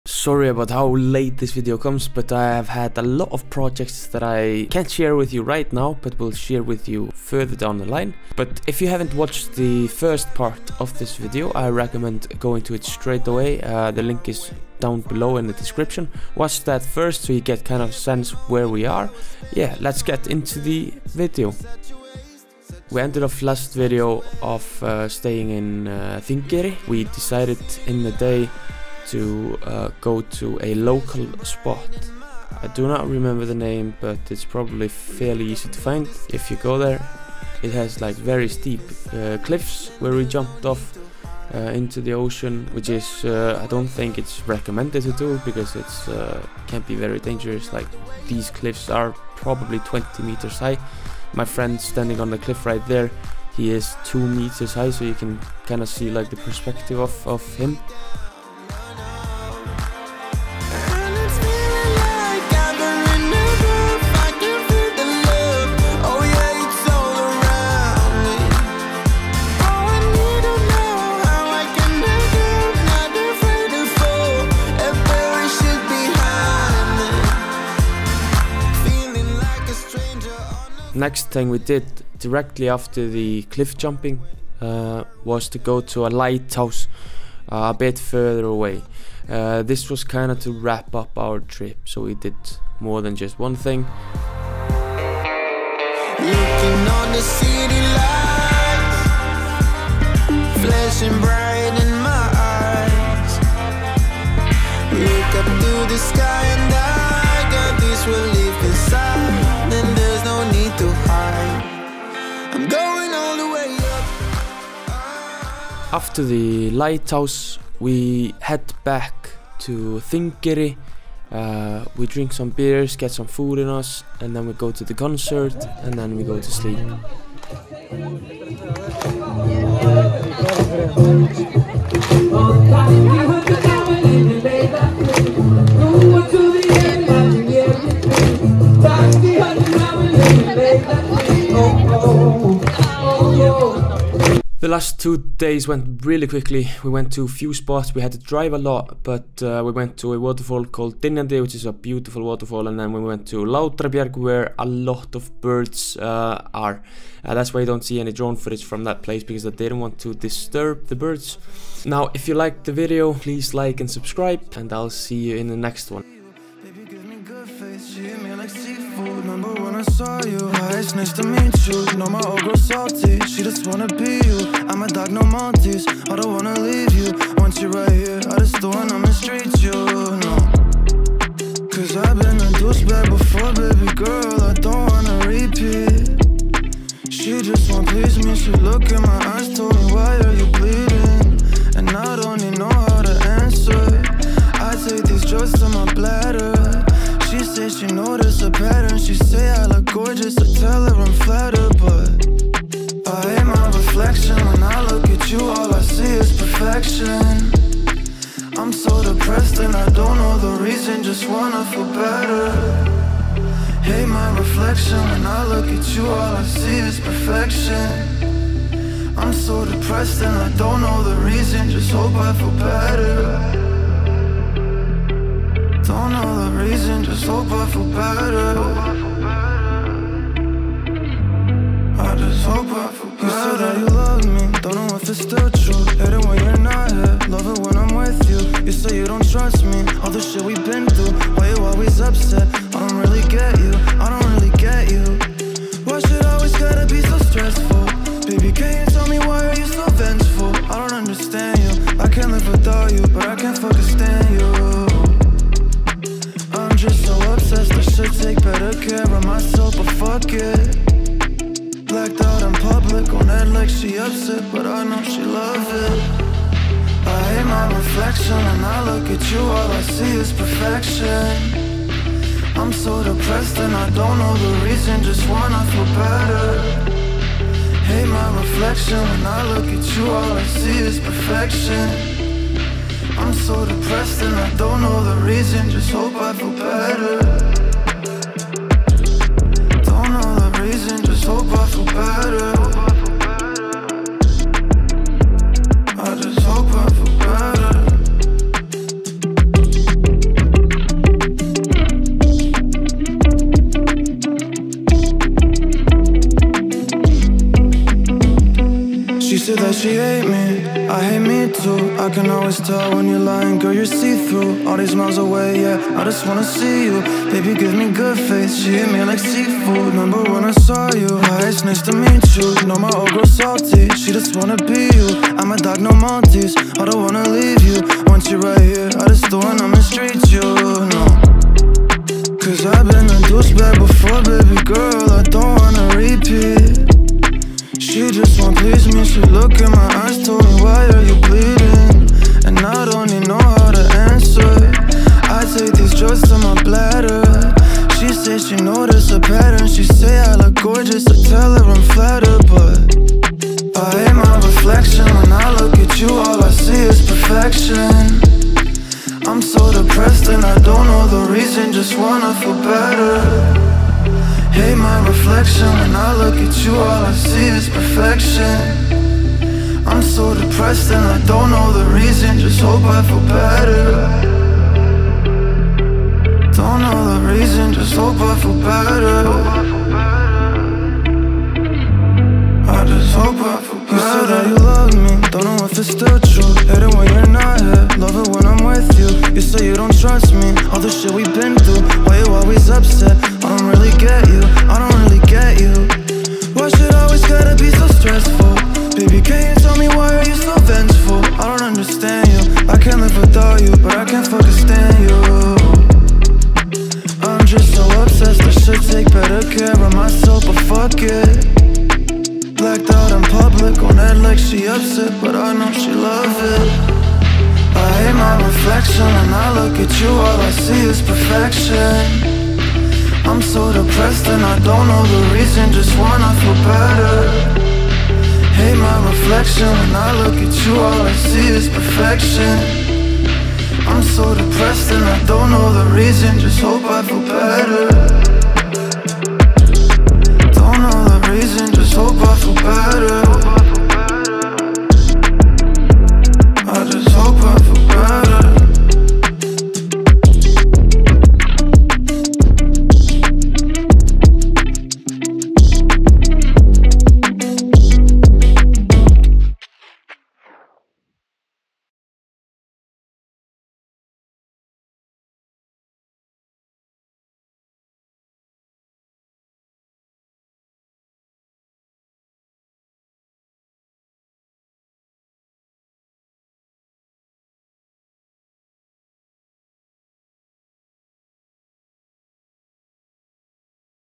Road trip around Westfjords in Iceland - Part 2/2
Take the journey with us through the westfjörds in Iceland on our 7-day road trip through the amazing landscape that this place has to offer!